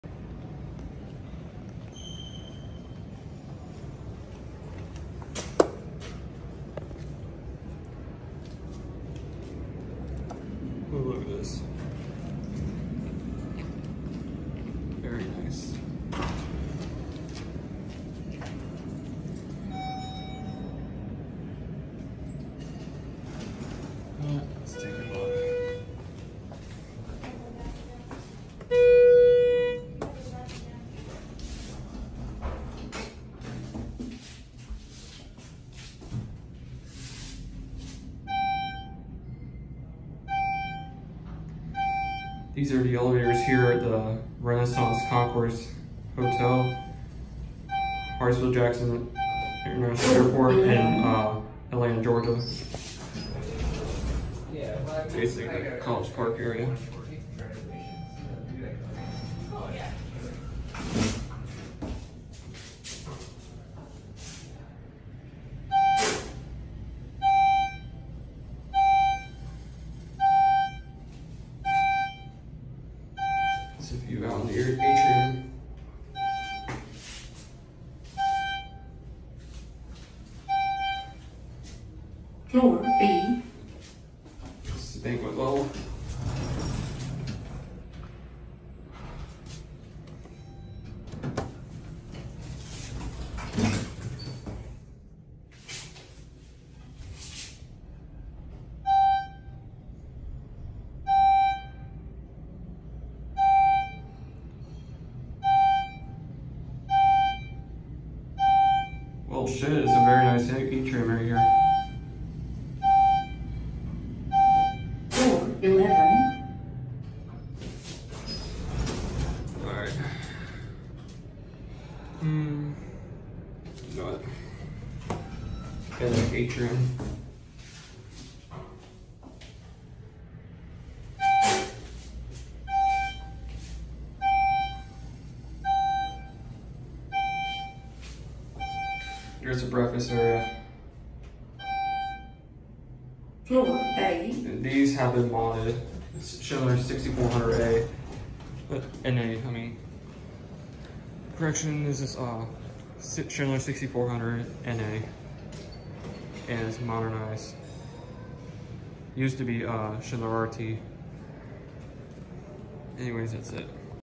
Tastefully Modded Schindler 6400NA Traction Elevators at Renaissance Concourse ATL, Atlanta GA
In my opinion I really love how the call stations are green and red and same thing with the lanterns

Brand: Schindler

Type: Traction

Fixtures: MAD BS moon style

Landings: 12

Speed: 350fpm

Doors: Center Opening

Capacity: 3500

Installed: Unknown

Modernized: 2019